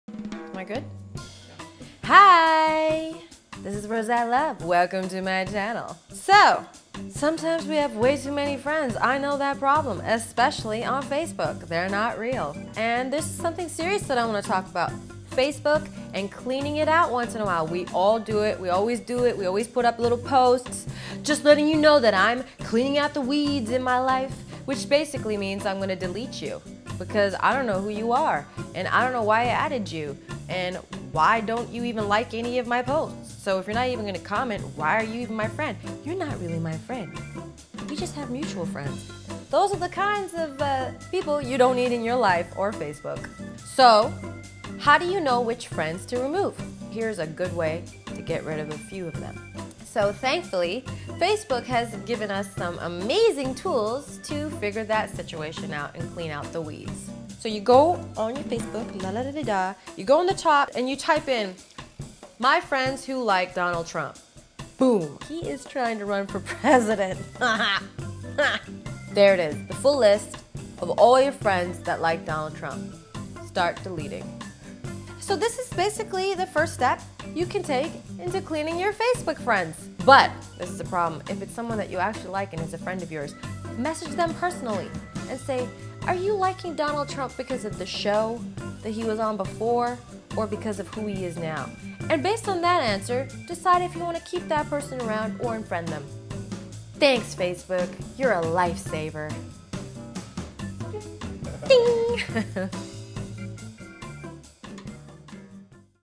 How to clean your Facebook friends list
Here’s a useful tip everyone can use to help clean their Facebook friends list.